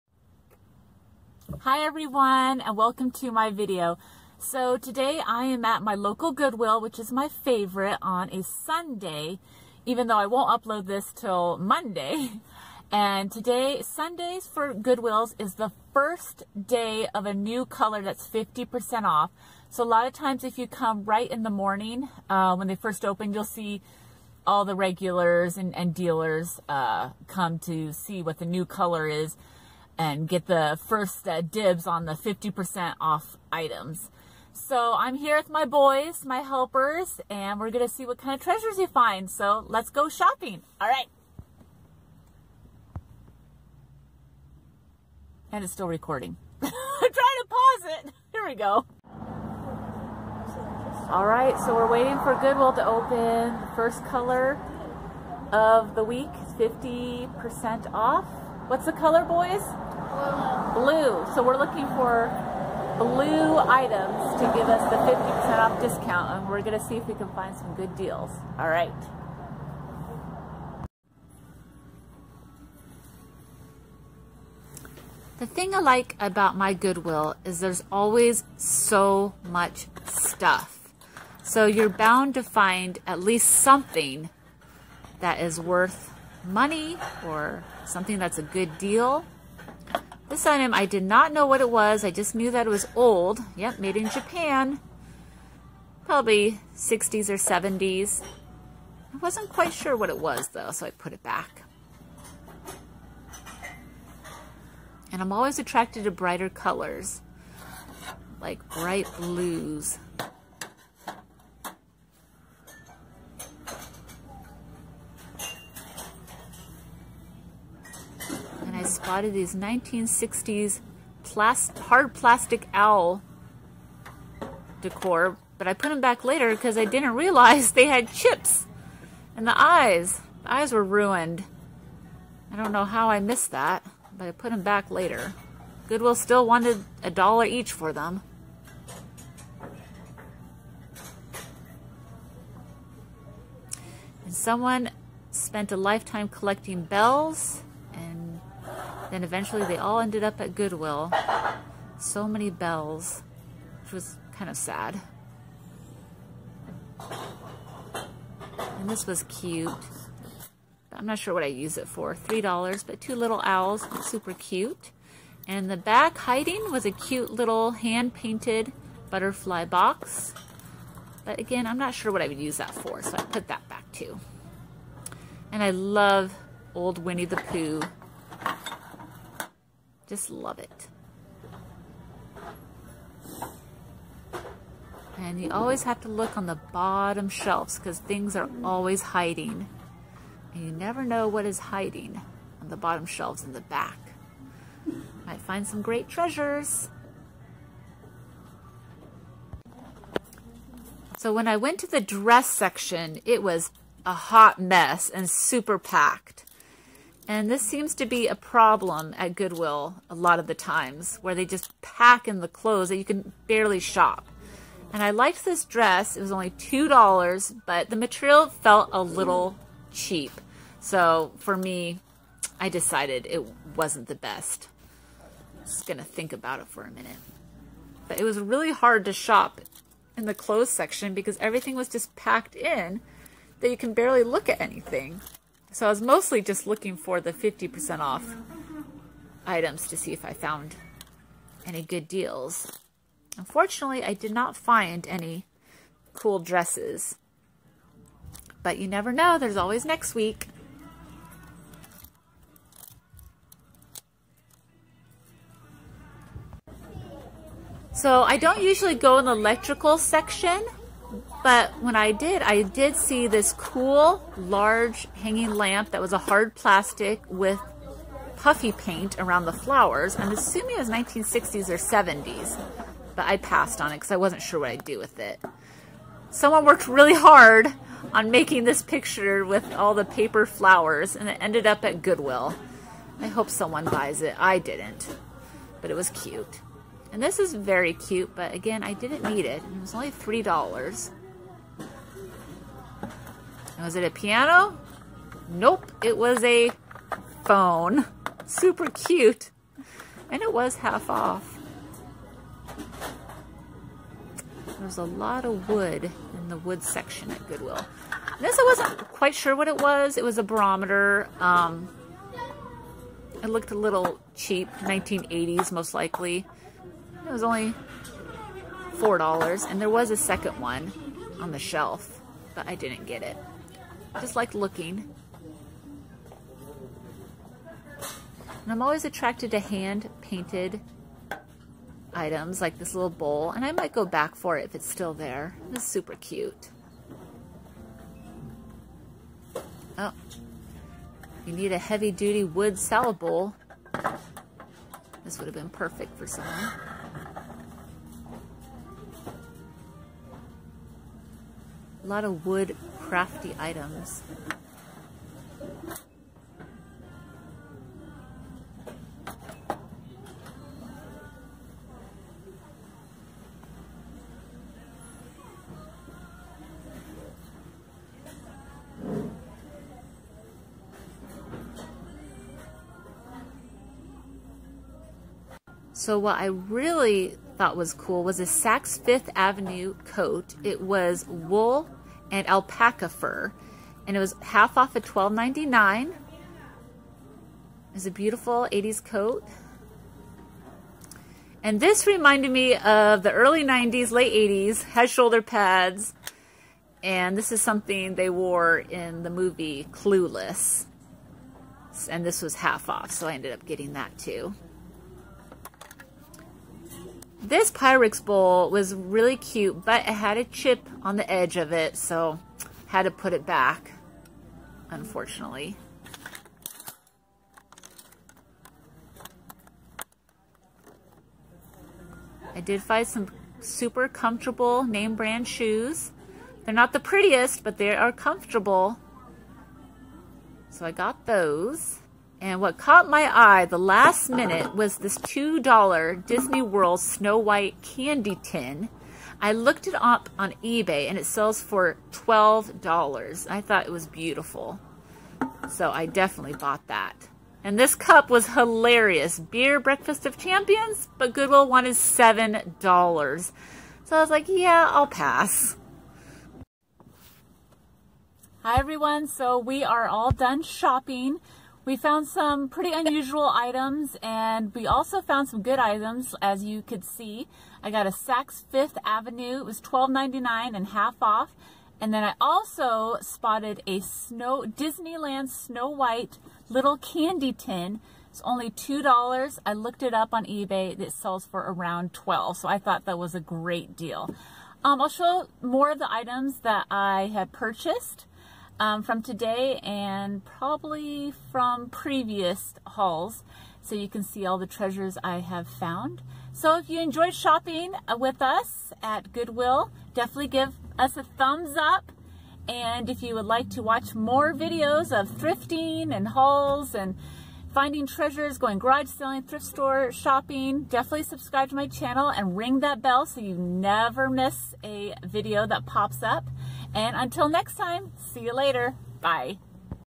Back to my favorite Goodwill! What did I find?
Shopping at my favorite Goodwill.  I got lucky and found some great items that I can sell on Ebay.  Check it out!